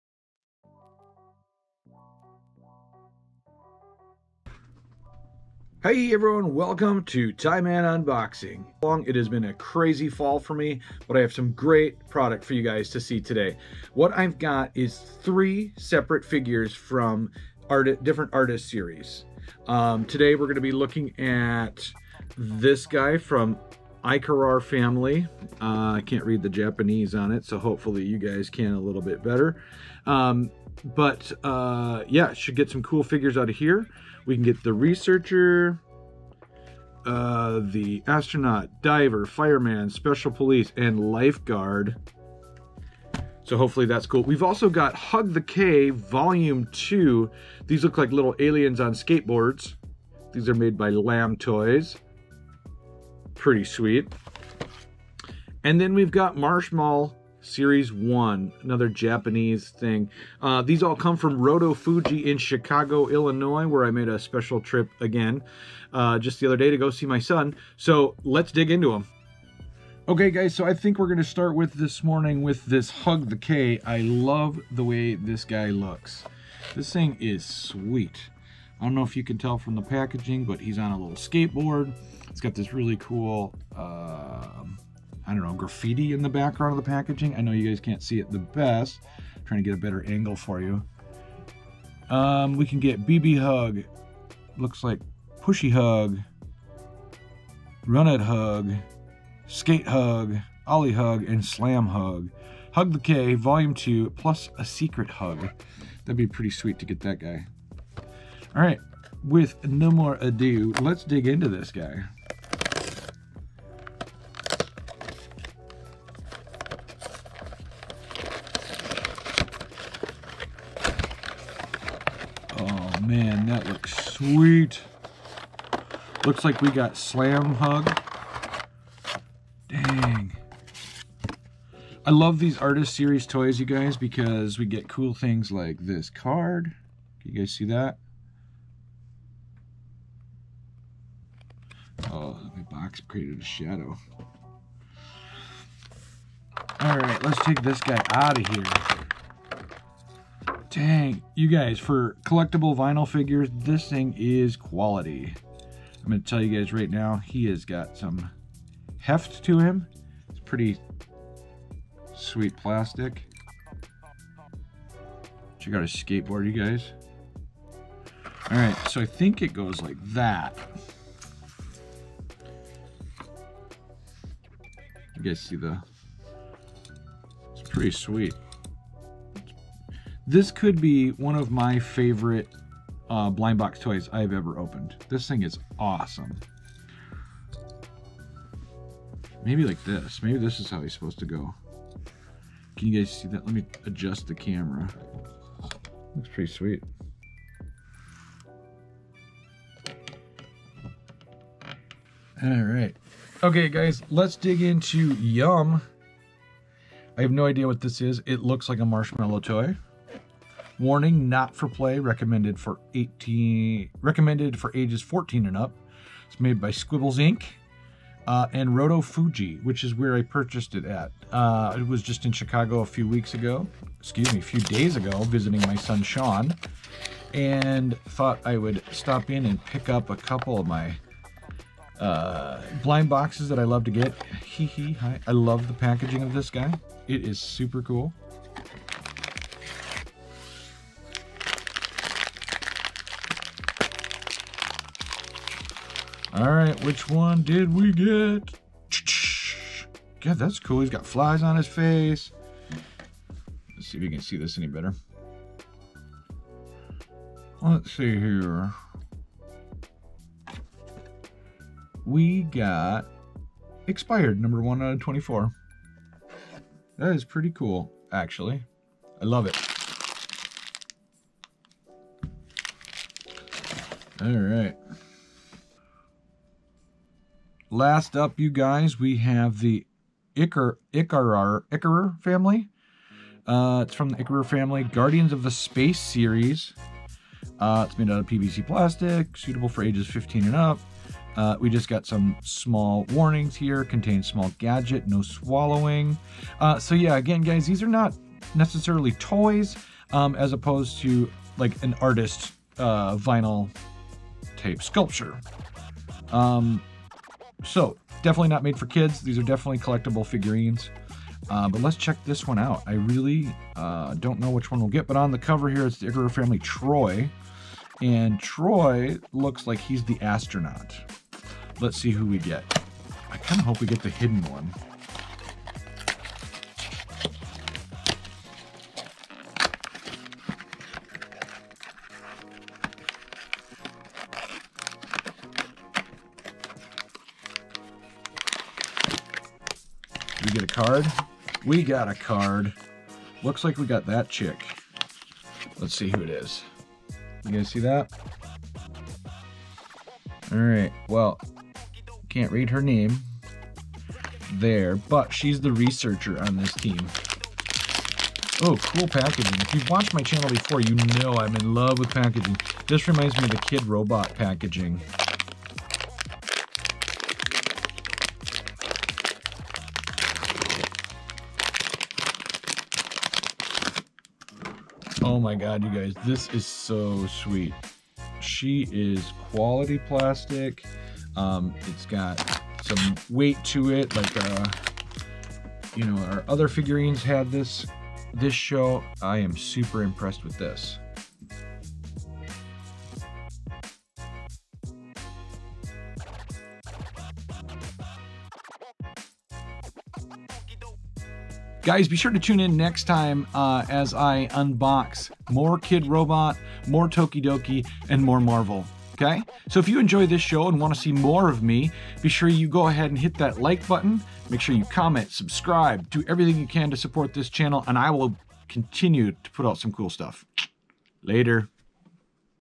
Ty-Man Unboxing Vinyl Art Toys
In this episode of Ty-Man Unboxing Tyler shares some of his recent Art Toy acquisitions.